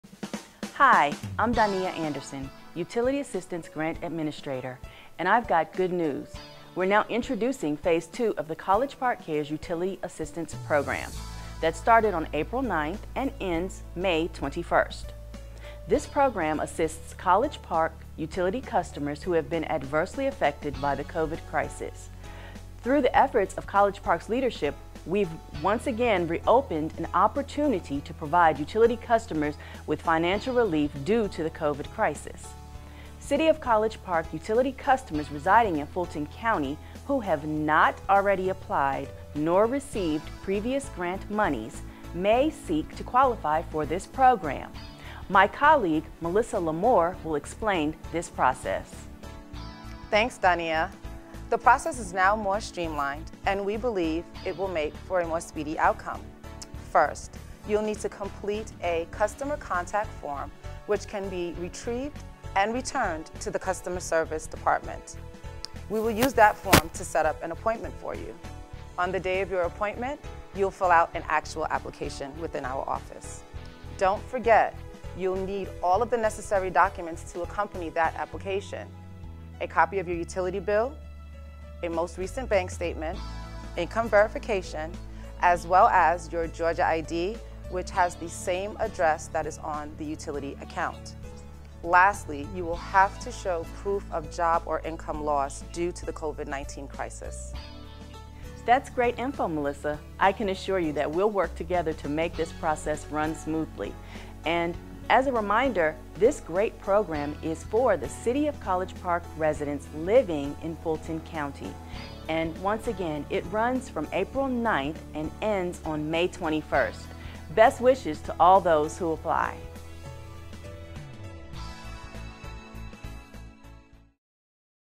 College Park, GA Utility Assistance - Covid 19 Crisis
Thanks to the efforts of College Park city leadership, utility assistance funds have once again been made available for City of College Park utility customers who reside in Fulton County, and have encountered financial hardships due to the Covid-19 crisis.  The video explains the application process.